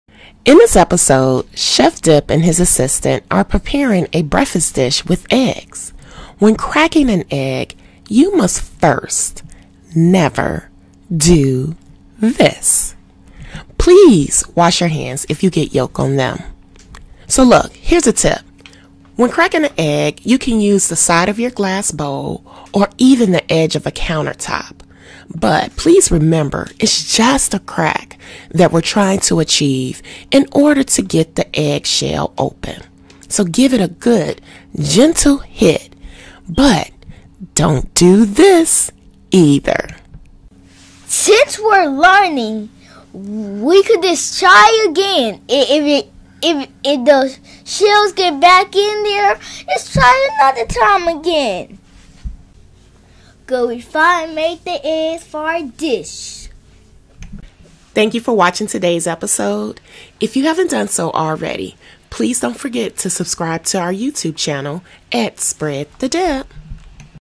Crack an Egg | Kids can cook - How to become a Junior Chef on Ep 1 The Dip Show
Chef Dip and his assistant cracks their first egg. Although, they are still learning the method!!!

In general, children will learn how to food, meals, and get around the (pretend) kitchen on the Dip Show. Other work by our guest will be showcased on the Dip Show.

* If you want to order a junior chef set, please go